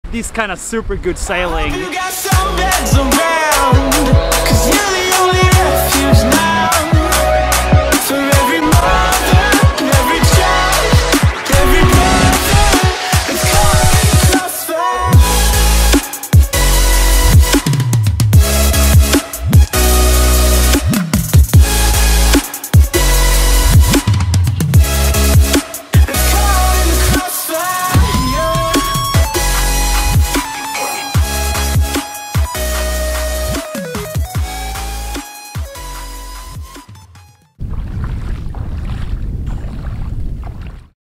Soon back onboard - can be BIG CHANGES - Season 5
We sailed 2500 miles after the launch last season. Let's see how many miles and what adventures we can get into this season. Our plan can change in favor for new adventures as well. but what? U might wanna come onboard and experience Hot Sauce while u can? Cheers!!